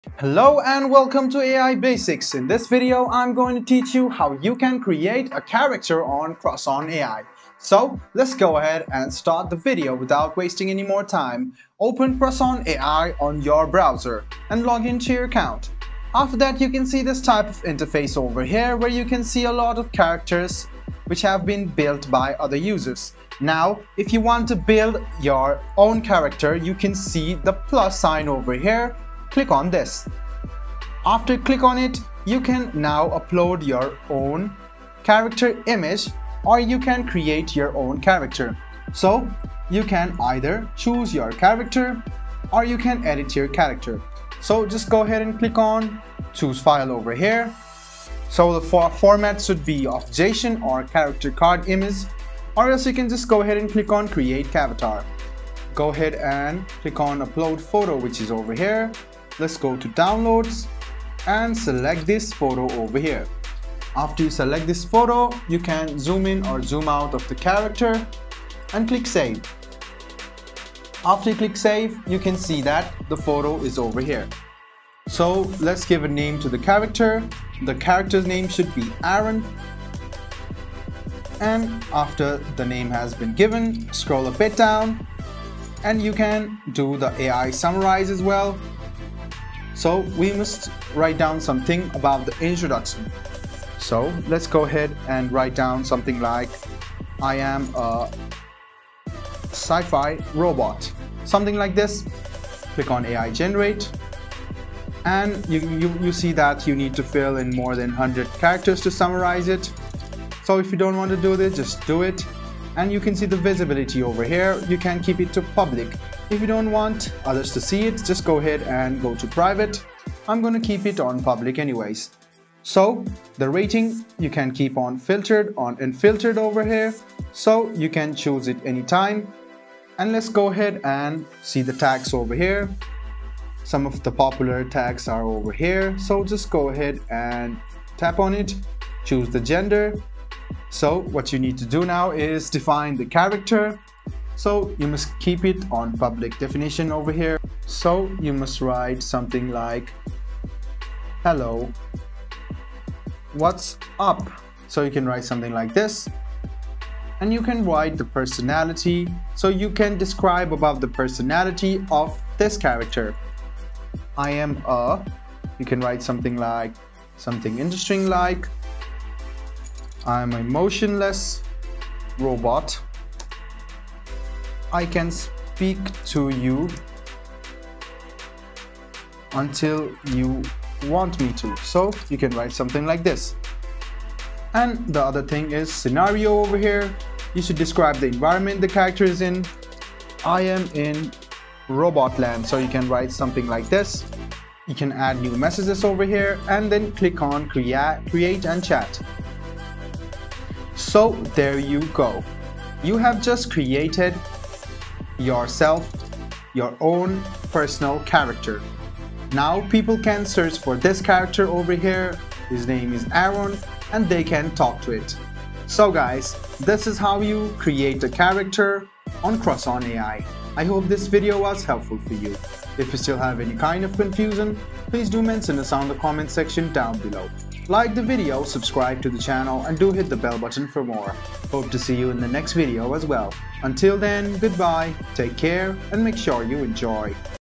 How to Create Character on Crushon AI (2024) | Register Character on Crushon AI
This tutorial covers selecting character traits, designing their appearance, and adding personality details to bring your virtual persona to life. Explore how to create and customize a character in Crushon AI for interactive role-playing experiences.